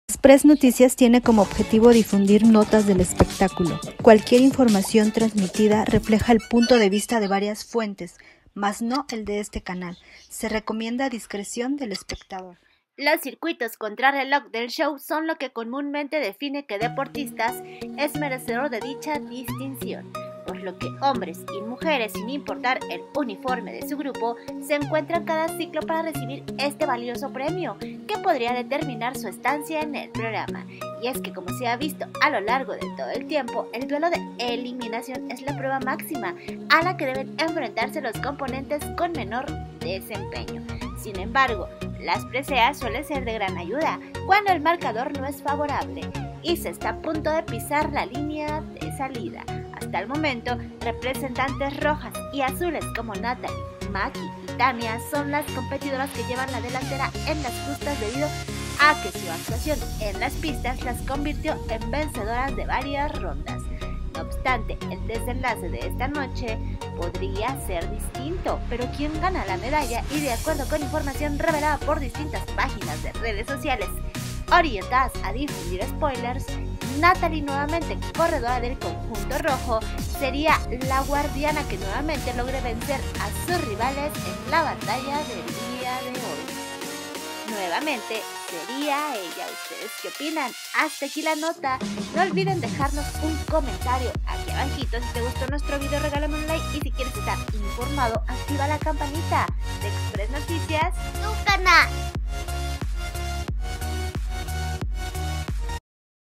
¿Quién gana la medalla femenil HOY miércoles 5 de enero en Exatlón México?
Te contamos qué chica conseguiría obtener la valiosa presea en los desafíos del reality.UNETE A NUESTRO CANAL HERMANO